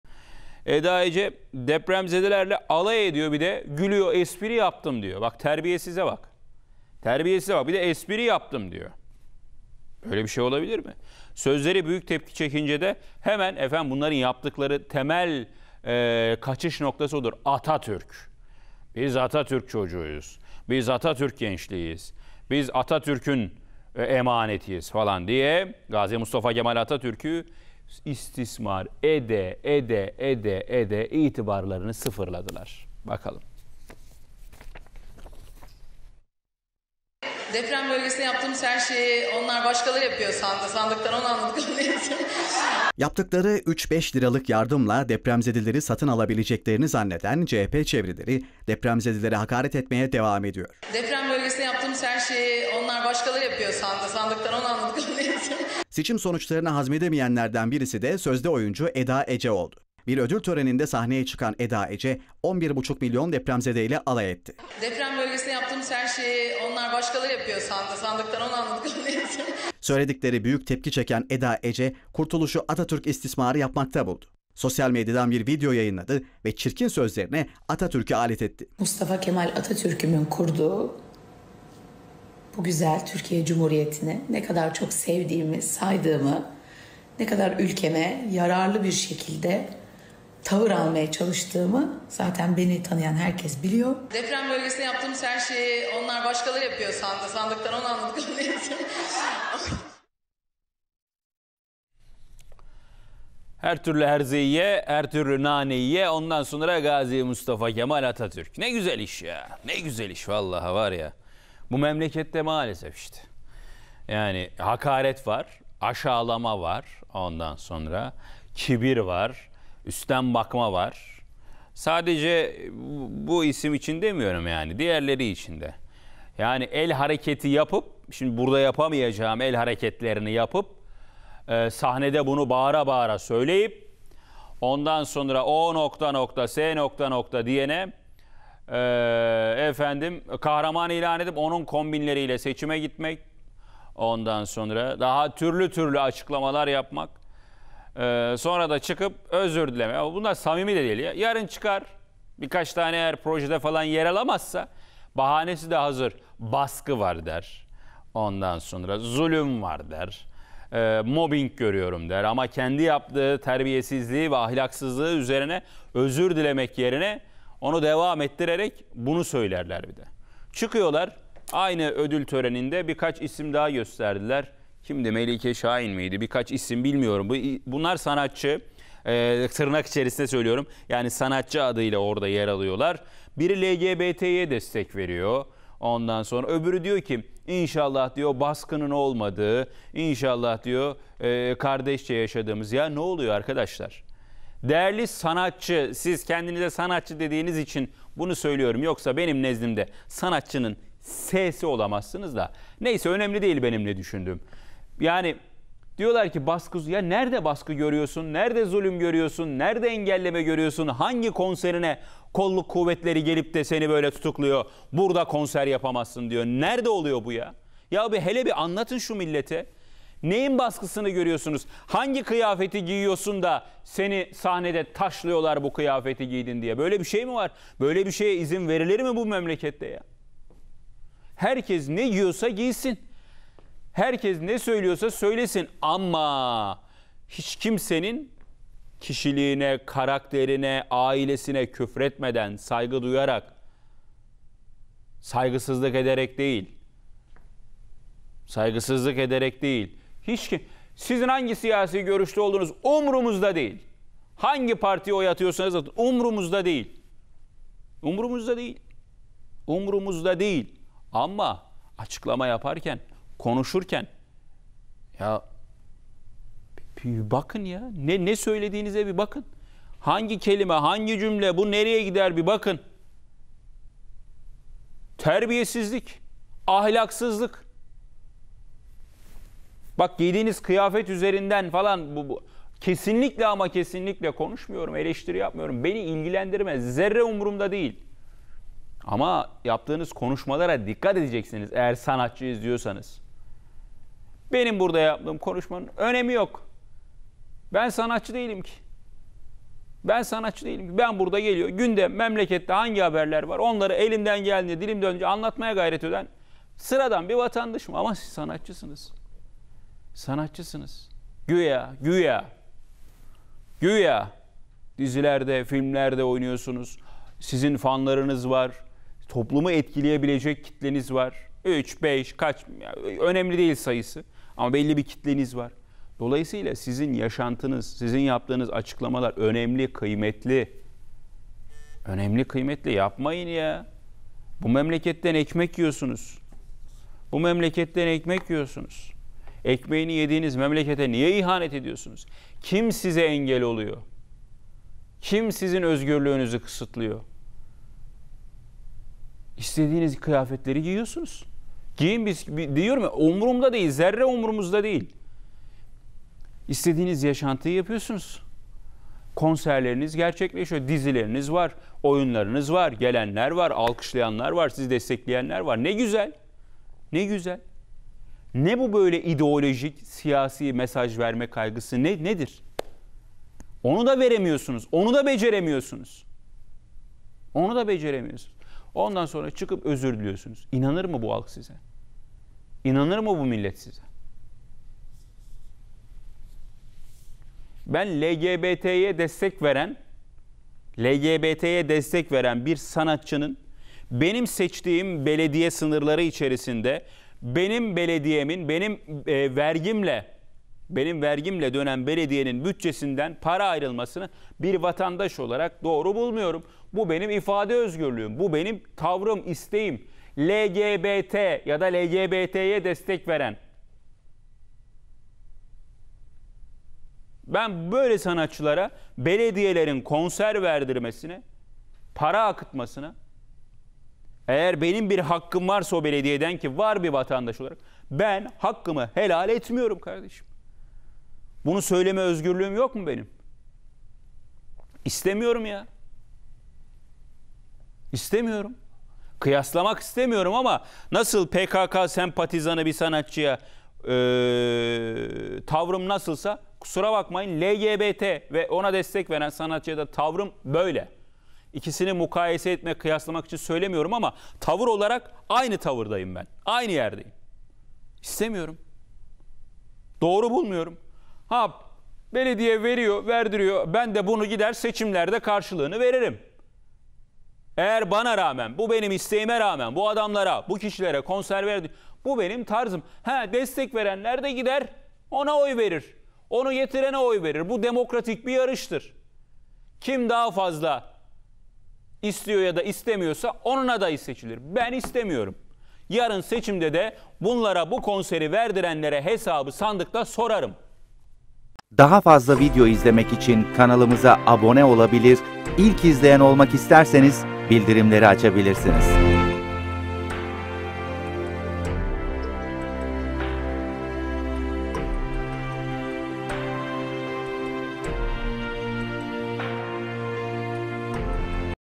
Eda Ece’nin Tepki Çeken Sözlerine Bir Yenisi Daha Eklendi! Sıkışınca: "Atatürk" Dedi
Bir ödül töreninde sahneye çıkan Eda Ece depremzedelerle alay etmişti. Olay yaratan sözlerin ardından Eda Ece, Gazi Mustafa Kemal Atatürk'ün ismini istismar ederek yaptığı ayıbı örtmeye çalıştı. Ödül törenindeki sözlerinin ardından özür dilemek yerine yaptığı Atatürk açıklaması daha çok tepki çekmesine neden oldu.